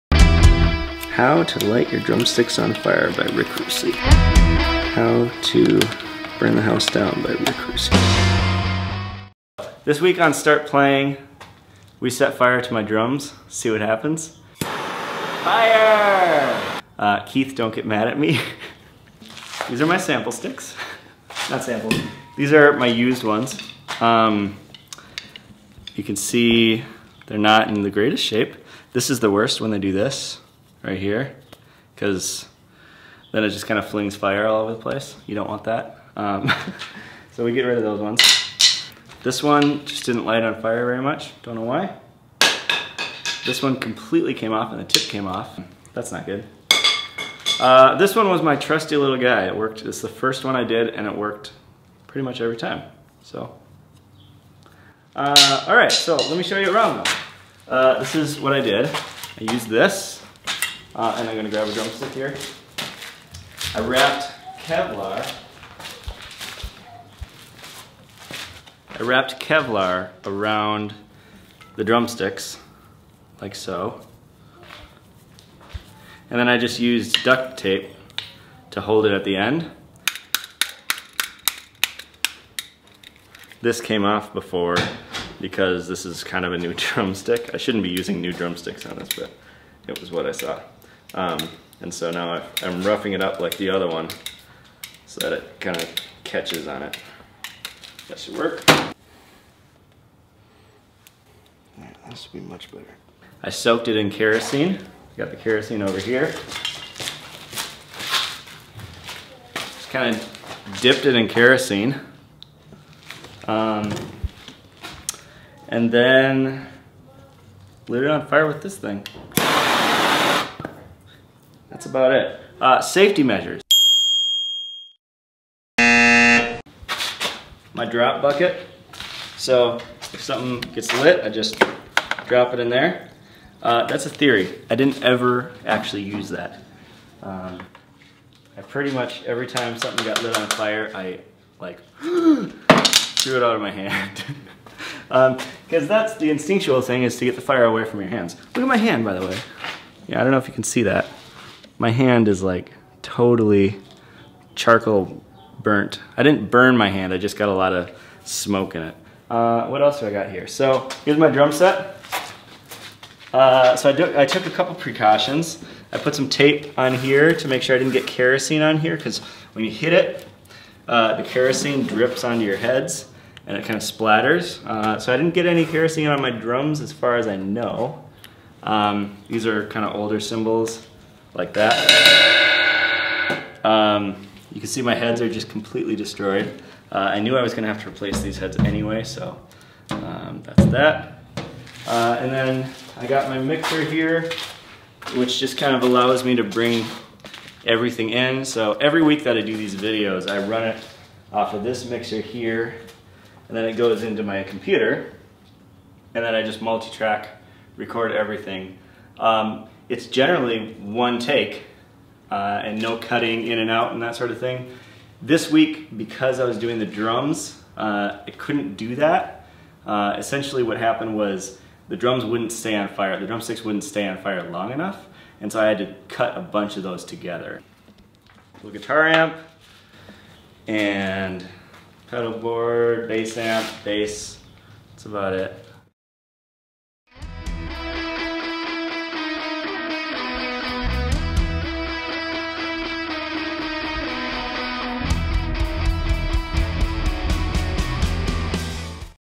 Behind the Scenes – Flaming Drums
Check out the making of for my Epic Flaming Drums video.

For you gear nerds:

- Canon t3i
- 50mm 1.8
- 50mm 1.4
- 18-55mm (kit lens)
- iPhone
- DaVinci Resolve 12 (free)

Audio:
- PreSonus StudioLive 16.0.4
- Audix D6 (Kick)
- Custom Built Subkick (Kick)
- SM57 (Snare)
- Audix D4 (Floor Tom)
- Audio Technica AT2020 (Overhead)
- Logic 9

Drums:
- Risen Drums Satin Maple // 22x17, 8x12, 12x16, 5.5x14
- Zildjian K Custom Fast Crash 17 and 18, 13" Custom Hybrid Hats

Guitar:
- Yamaha Semi-hollow (nothing special)
- Line6 Pod X3 Live
- Peavey Classic 30

Bass:
- Mexican P Bass (direct)